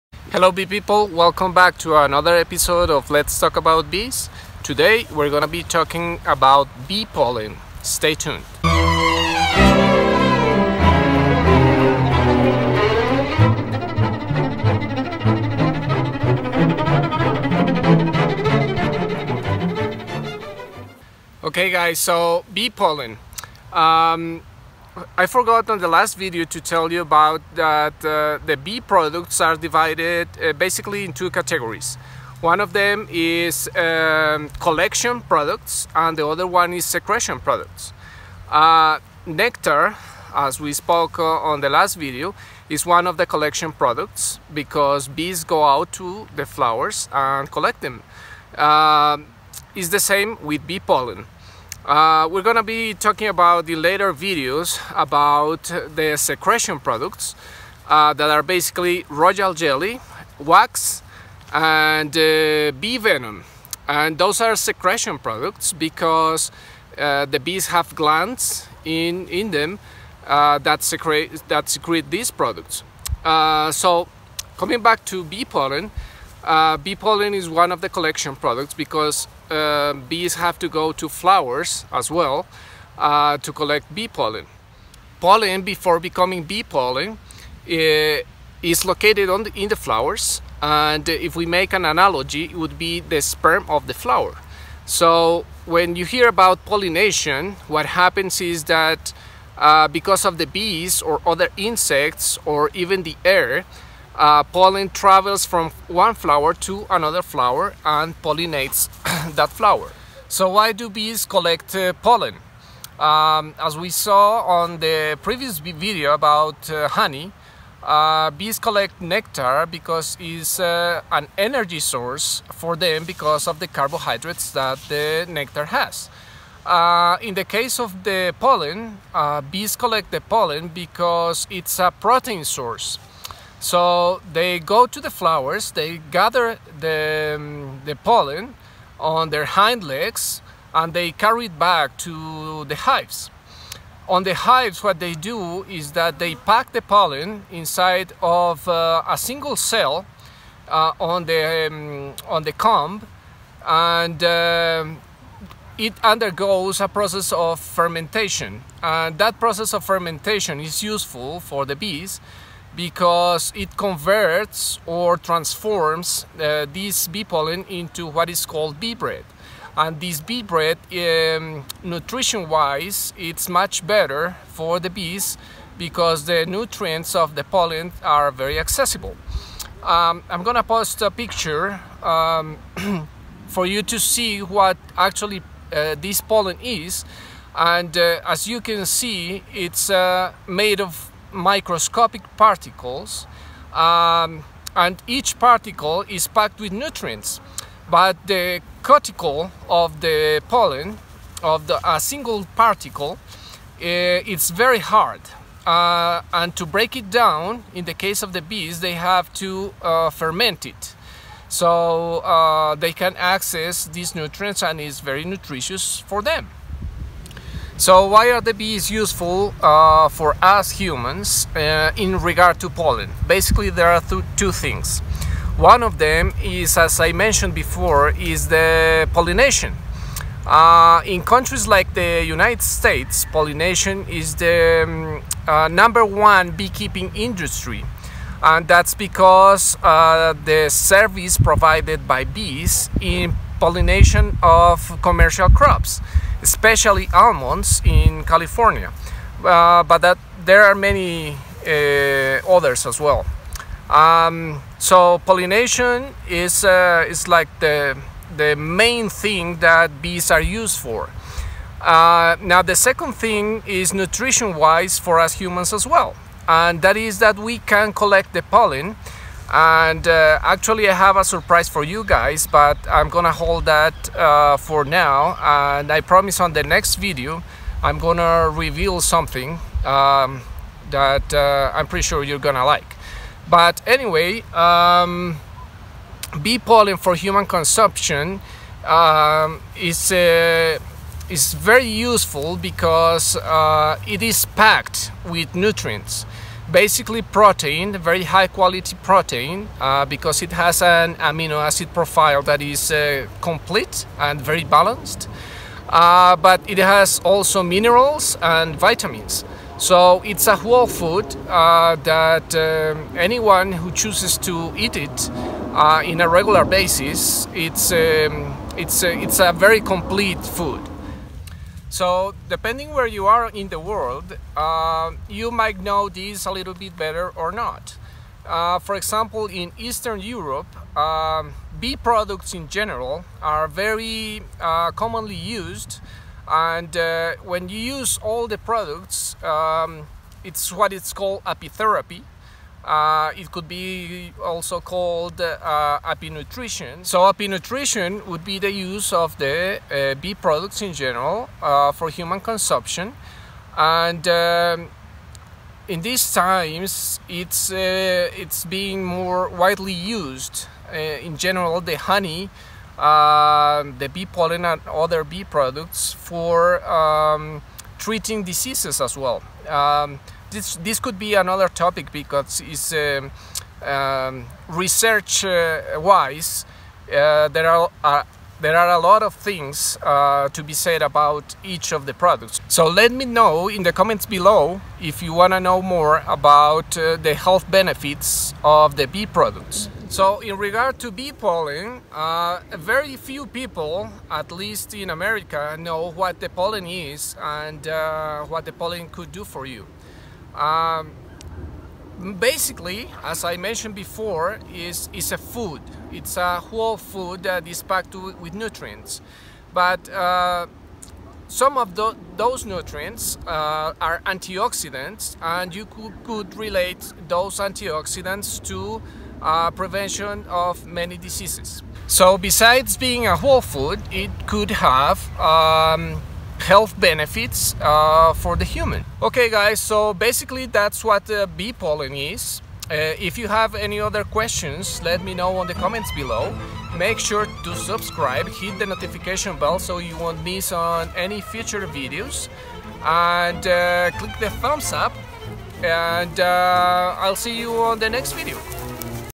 Amazing facts about Bee Pollen
In this episode we describe what bee pollen is, where does it come from, why the bees collect it, how they process it, why are bees important in regard to pollination, and pollen as food for us humans.

Performed by: The US Army Band